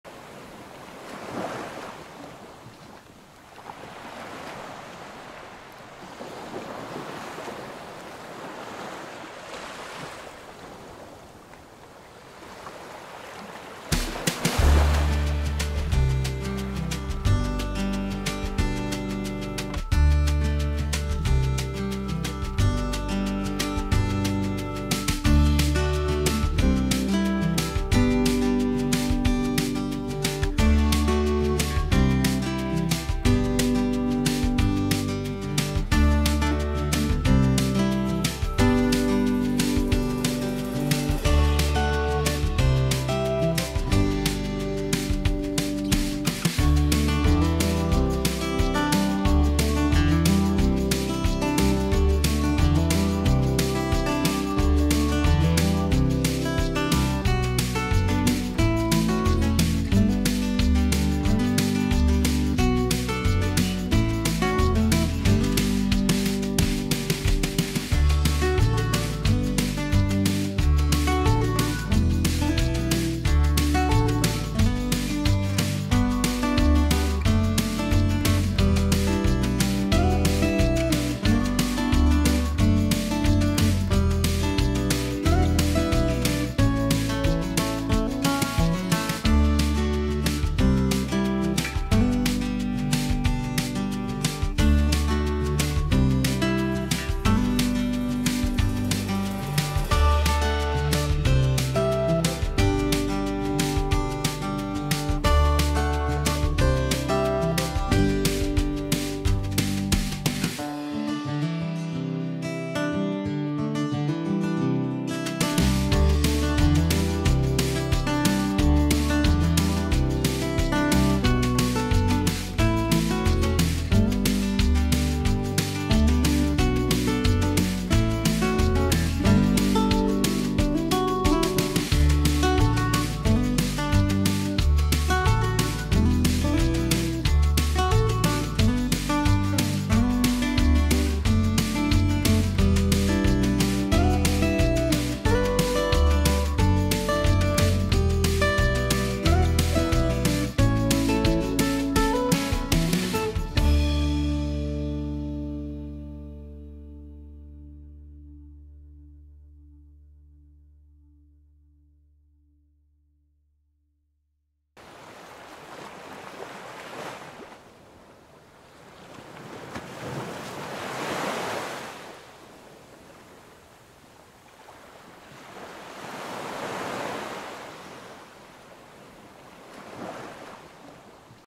Summers and beaches in Belize, Umaya Resort | FPV
On a recent weekend I stood up before the majority of the other guests at this resort and took my drone for a flight around the buildings, beach and pool. It truly was a lovely place for a weekend.